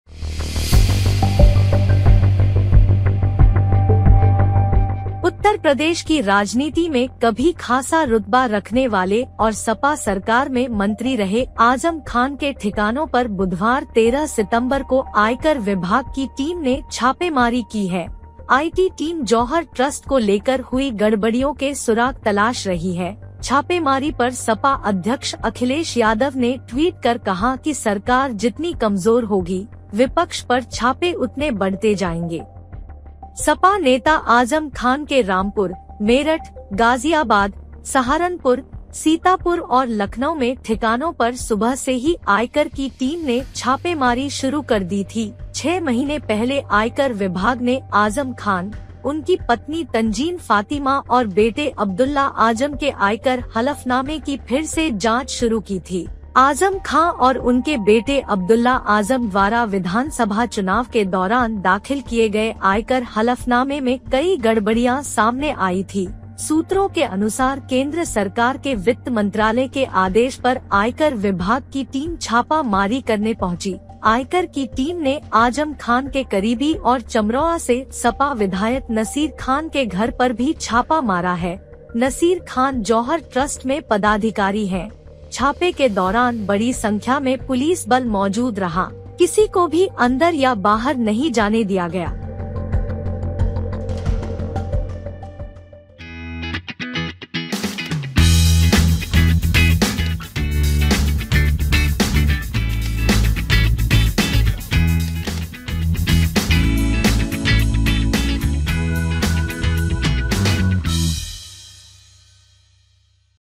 आजम खान पर आई एक और मुसीबत, अखिलेश यादव ने कहा- ये तो होना ही था...!
उत्तर प्रदेश की राजनीति में कभी खासा रुतबा रखने वाले और सपा सरकार में मंत्री रहे आजम खान के ठिकानों पर बुधवार13 सितंबर को आयकर विभाग की टीम ने छापेमारी की है। आईटी टीम जौहर ट्रस्ट को लेकर हुई गड़बड़ियों के सुराग तलाश रही है। छापेमारी पर सपा अध्यक्ष अखिलेश यादव ने ट्वीट कर कहा कि सरकार जितनी कमजोर होगी, विपक्ष पर छापे उतने बढ़ते जाएंगे।